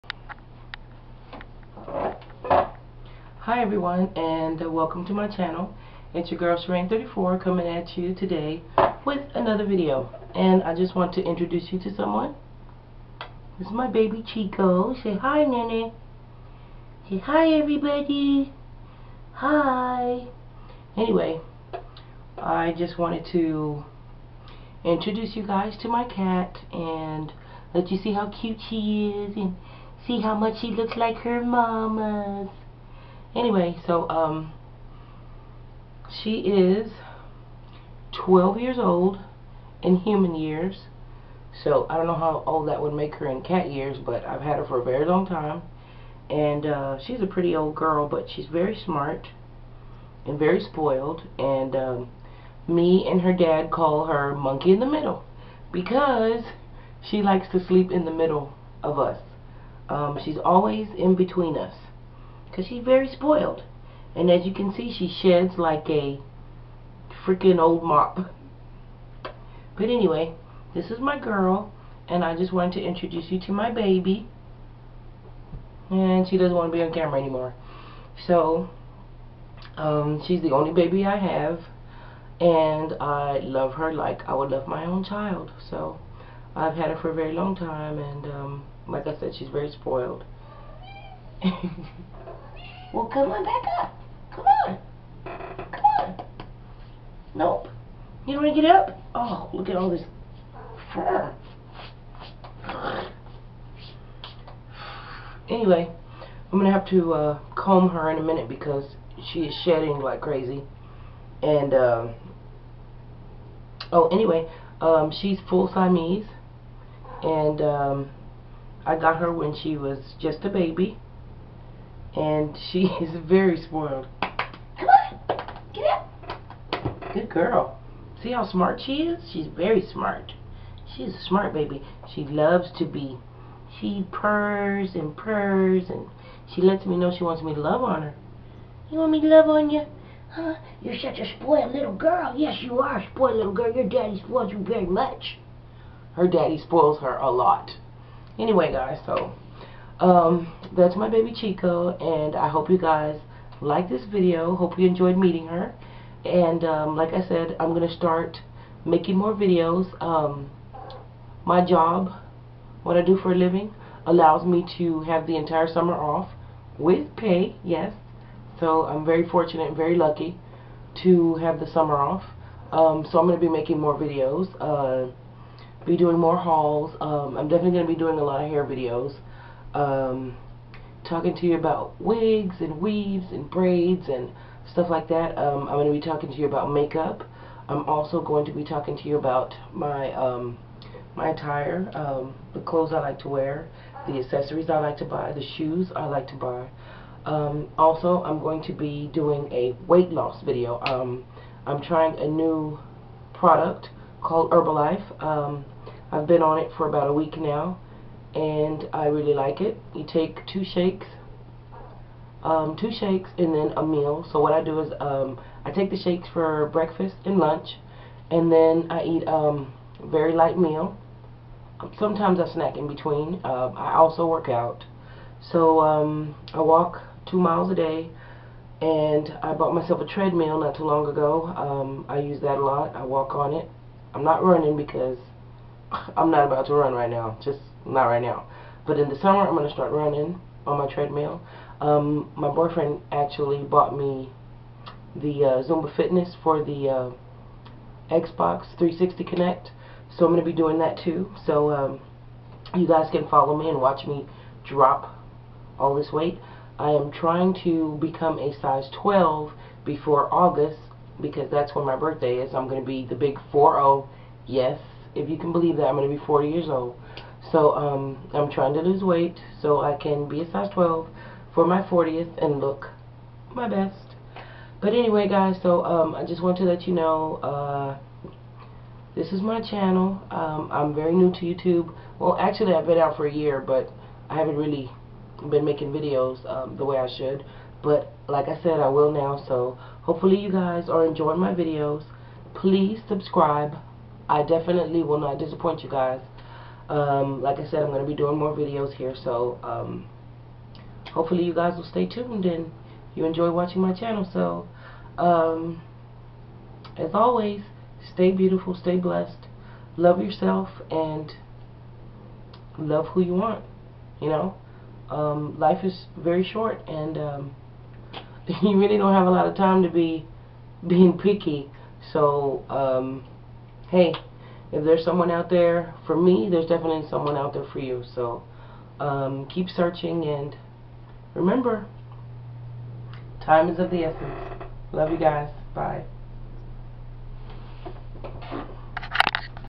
Weightloss with Herbalife, meet my cat Chico & all about my channel.
In this video, I introduce you to my cat Chico. I also discuss my take on Herbalife & how it's going to help me drop weight & I tell you all about my channel & what I hope to accomplish with it. (Please excuse the sweating. I had alot of lights on me) Hope you guys enjoy this video.